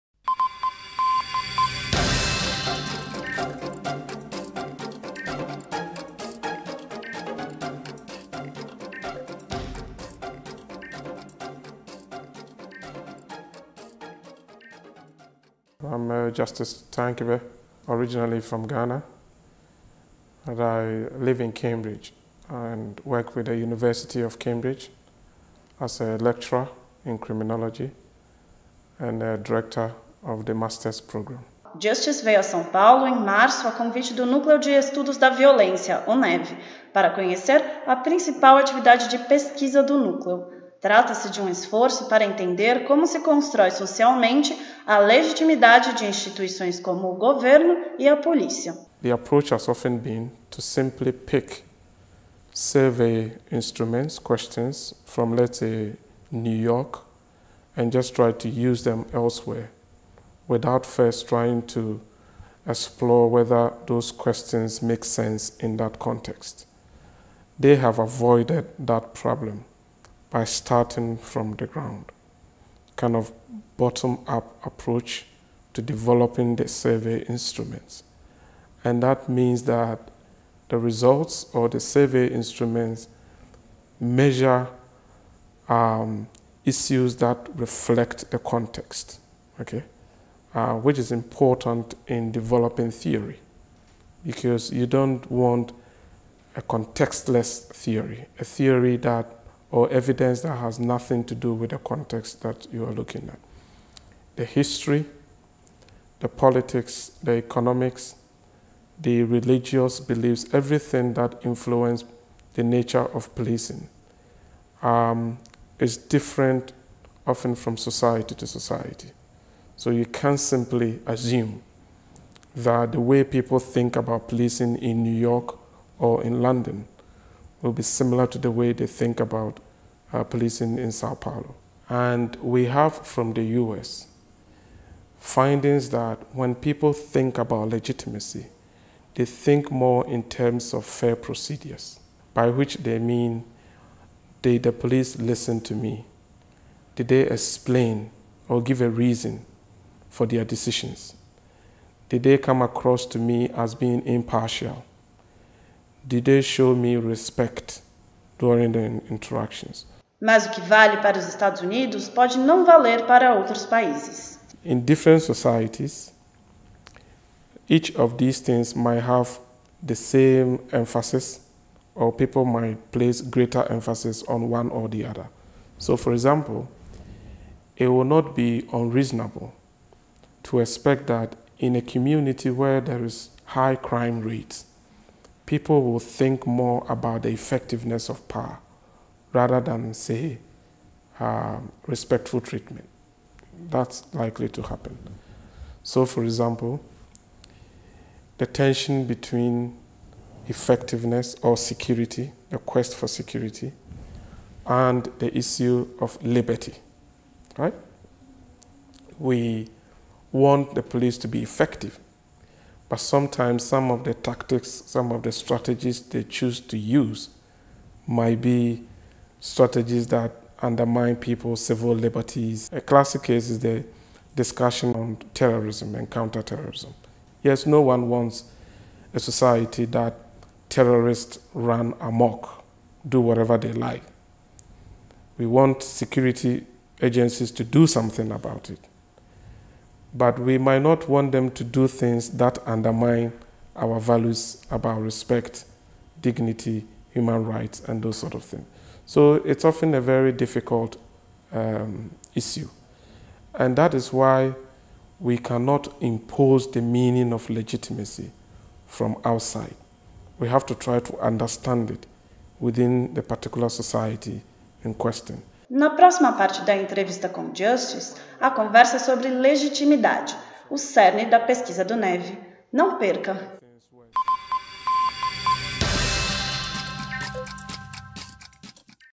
Pesquisador de Cambridge visita núcleo da USP
Justice Tankebe, professor de criminologia da Universidade de Cambridge (Reino Unido), veio a São Paulo para conhecer o trabalho do Núcleo de Estudos da Violência (NEV) da USP. A visita foi feita em março. Em entrevista ao Núcleo de Divulgação Científica da USP, ele comenta a abordagem adotada pelos pesquisadores brasileiros para investigar como se constrói no dia-a-dia a legitimidade das instituições públicas, como a polícia.

***ATIVE AS LEGENDAS EM PORTUGUÊS PARA MELHOR COMPREENSÃO DO VÍDEO***